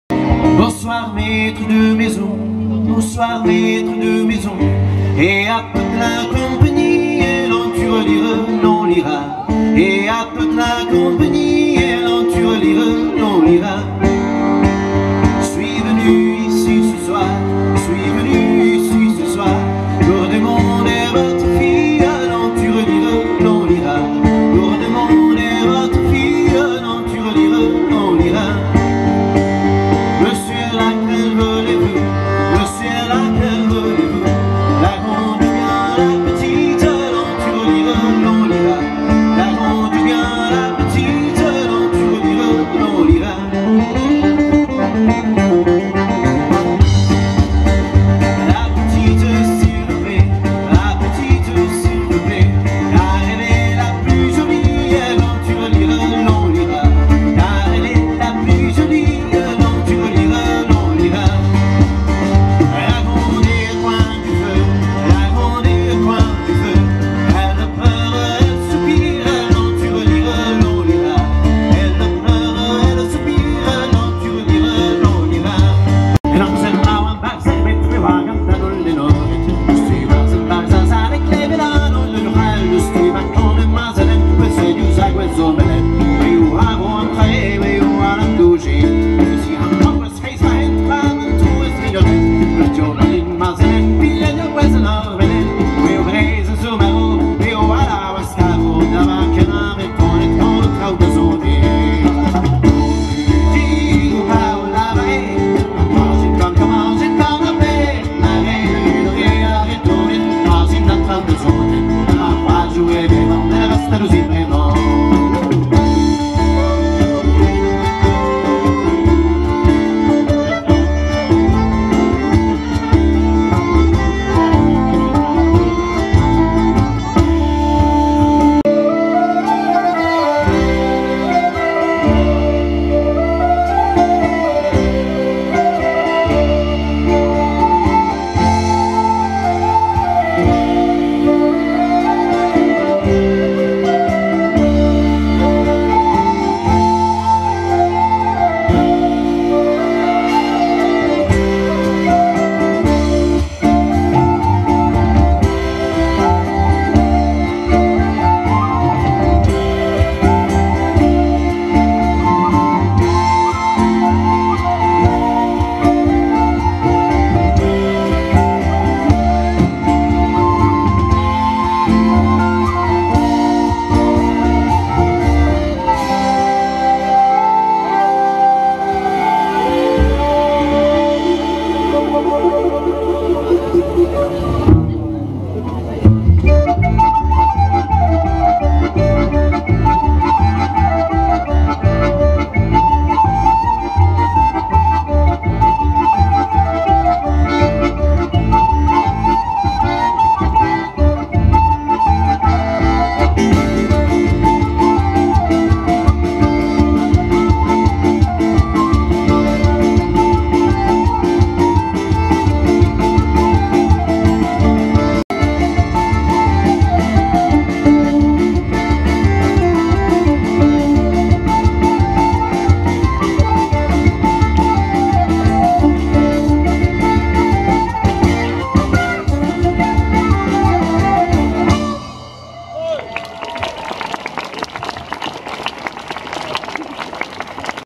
Svatohorská šalmaj 2016 - ukázka z vystoupení skupiny BRAN
- Ukázka písničky ve francouzštině a bretonštině a na závěr krásná instrumentálka☺.
- En Esperanto: Specimeno el prezentiĝo de la ĉeĥa muzikgrupo BRAN okupiĝanta pri muziko de la antikva Bretonio.
Mallongigo de kantoj en la franca, en la bretona kaj fine bela instrumenta muziko. - Registrite dum la kultura plenaera festivalo Svatohorská šalmaj, okazanta ĉiujare en septembro en la pilgrimloko Svatá Hora (Sankta Monto) apud Příbram (sude de la ĉefurbo de Ĉeĥio Prago).